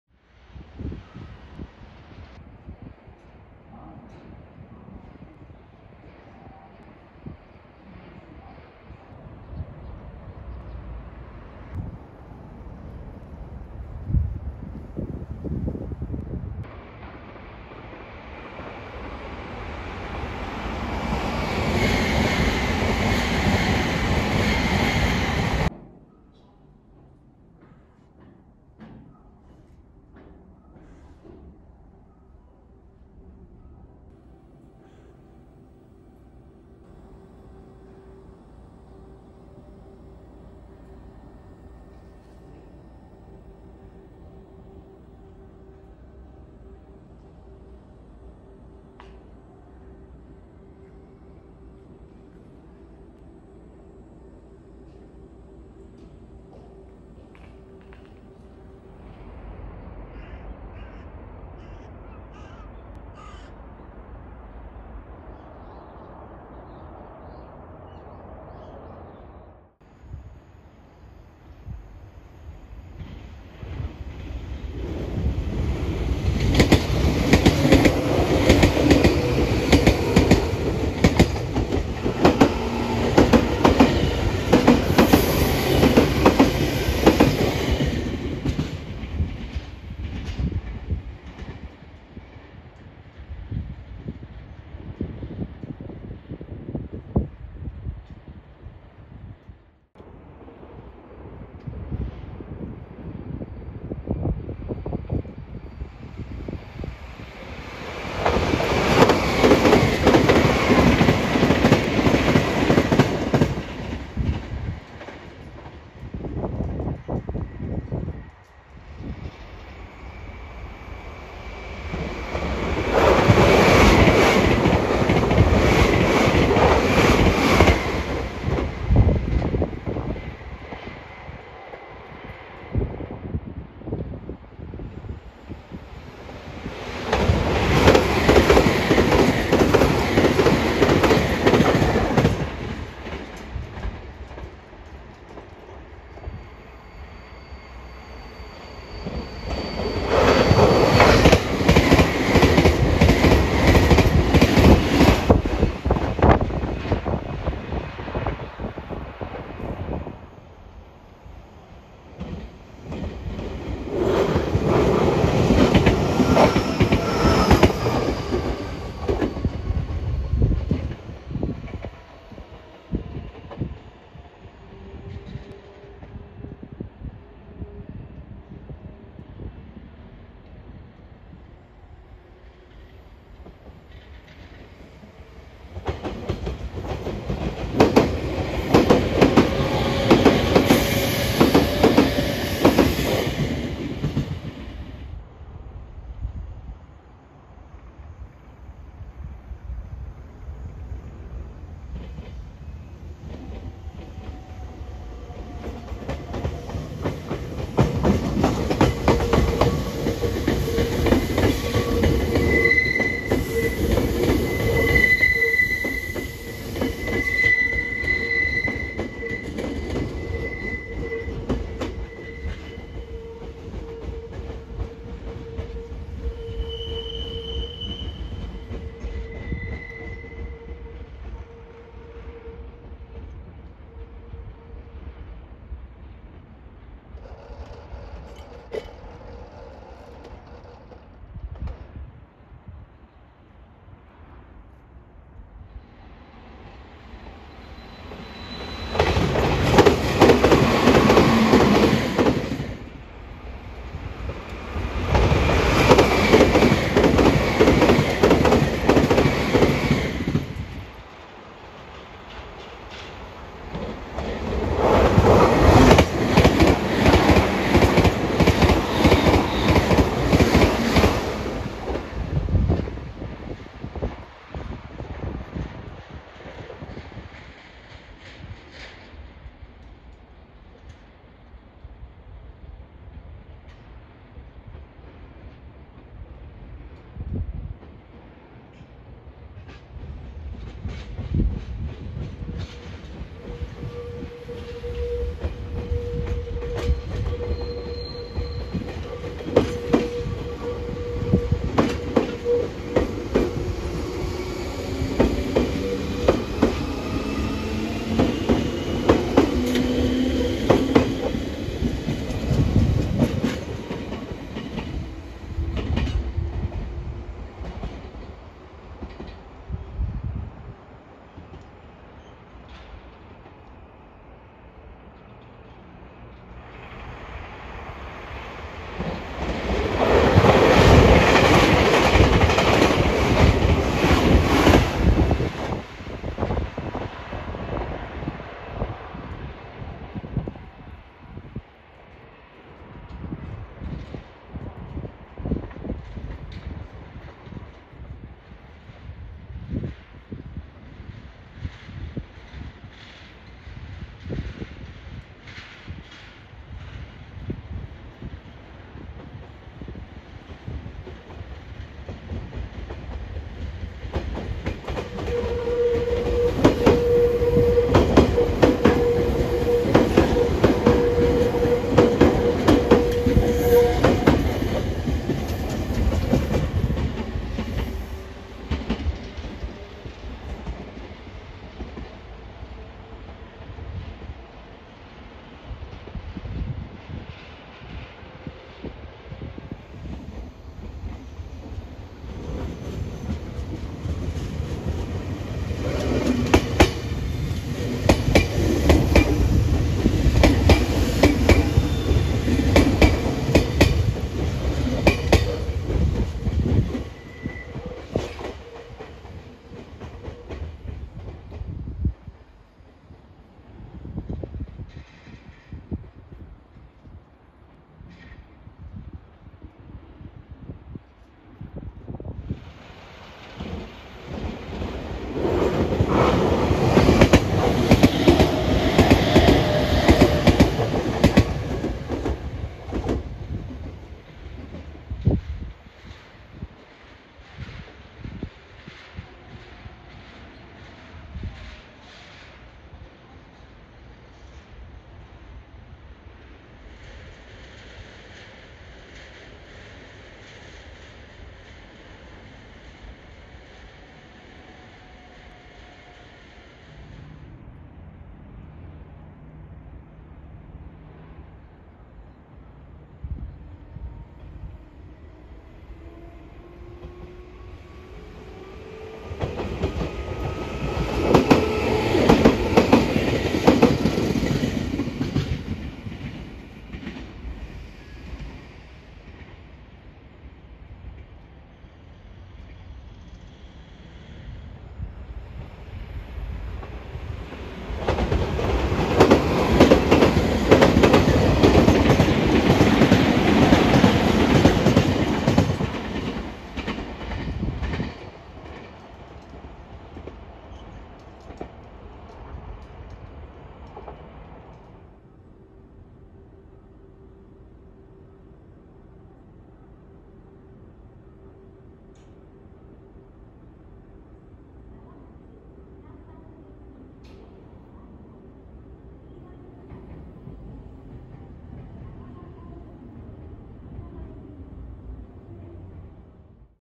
【駅ネタ⁉】どえらいデカい無人駅⁉名鉄 豊明駅 Japan's largest unmanned station⁉ Meitetsu Toyoake Station
2020年12月6日　名鉄名古屋本線豊明駅にて
iPhoneで撮影
国内最大の無人駅豊明駅を撮影
規模の大きさと駅の静寂のギャップが名鉄って感じです
December 6, 2020 at Toyoake Station on the Meitetsu Nagoya Main Line
Taken with iPhone
Photographed Toyoake Station, the largest unmanned station in Japan
The gap between the scale and the silence of the station is like Meitetsu.